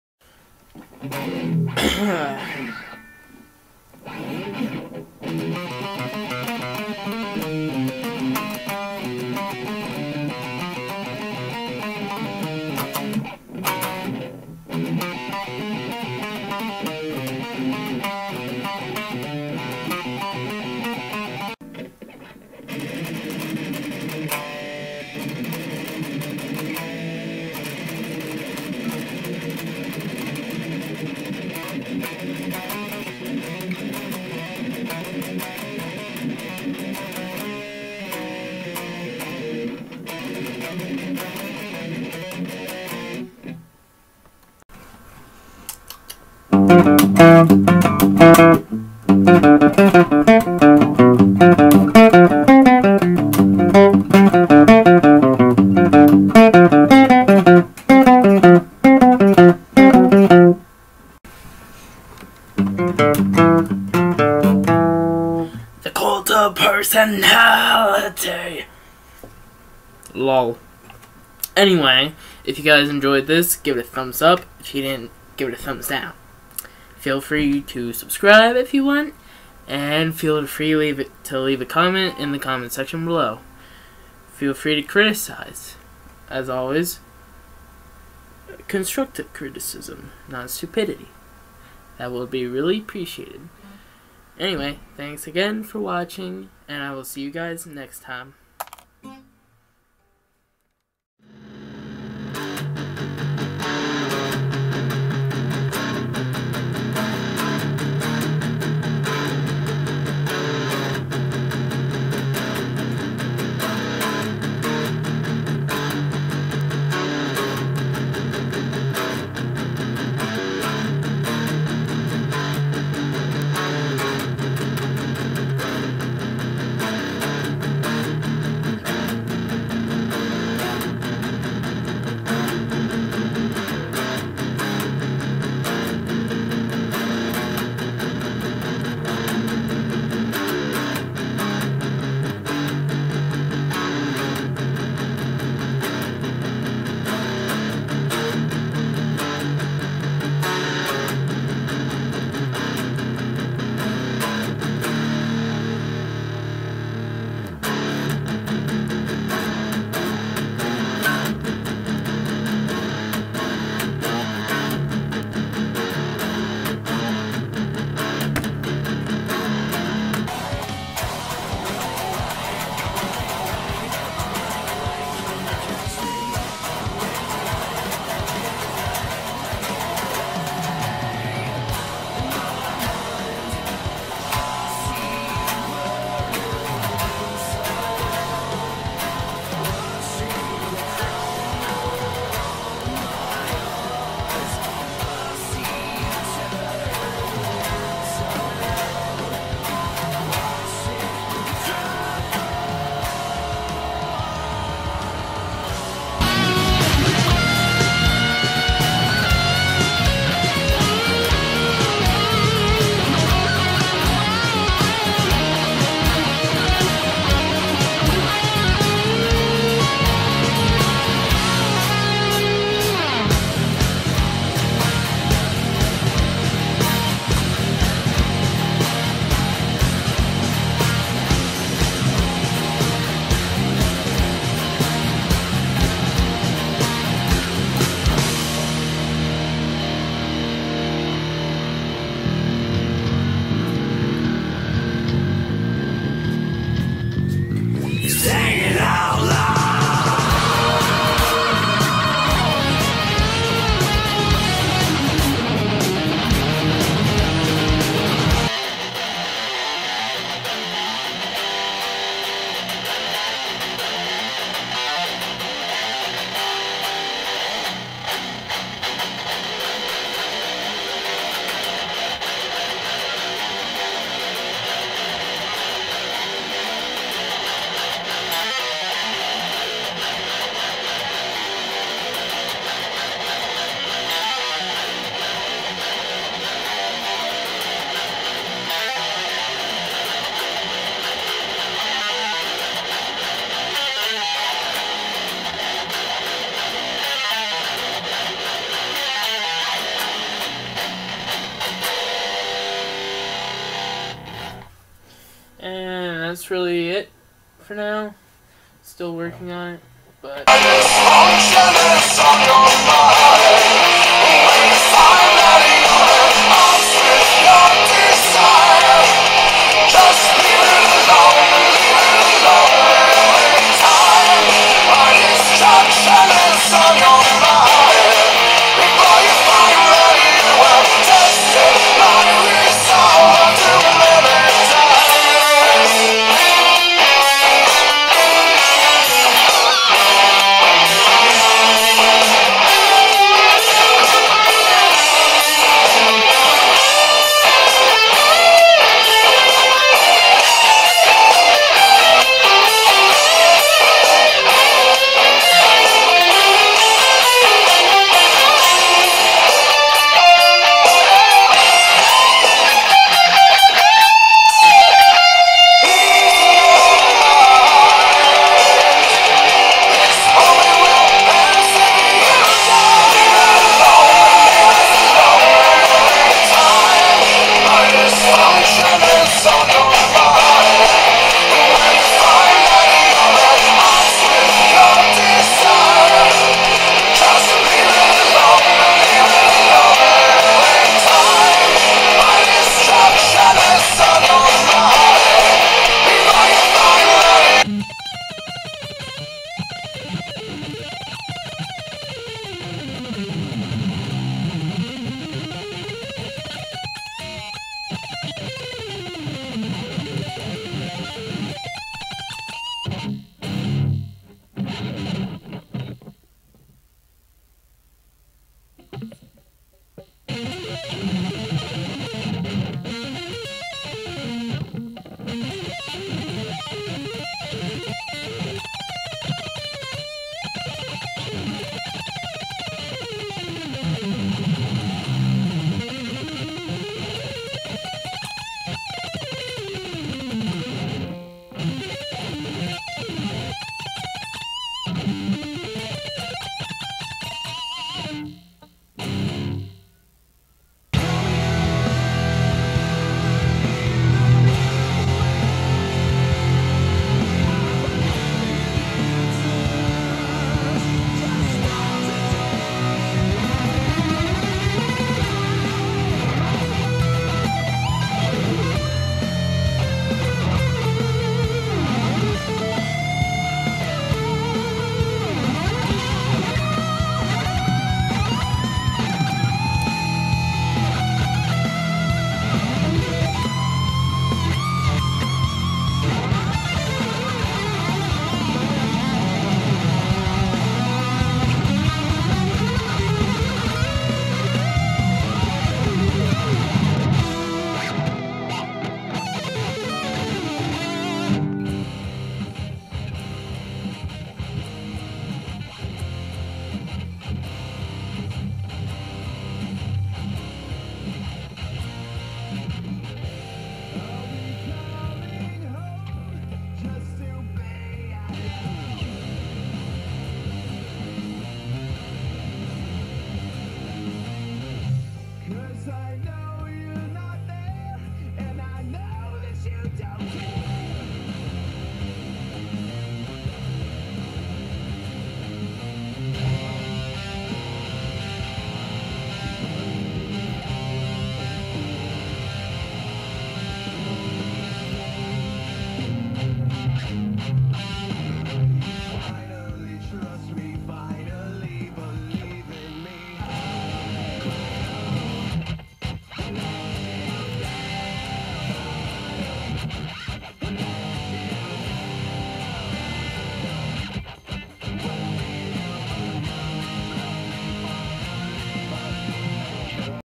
2015 - 2017 Compilation | Covers/Original Riffs
Sooo, yeah. The title pretty much sums it up. This is just a compilation of some of my videos from 2015 to this year. I think I've improved some. Anyways, thank you guys for watching this cringe-worthy video. Stay heavy! \m/ :)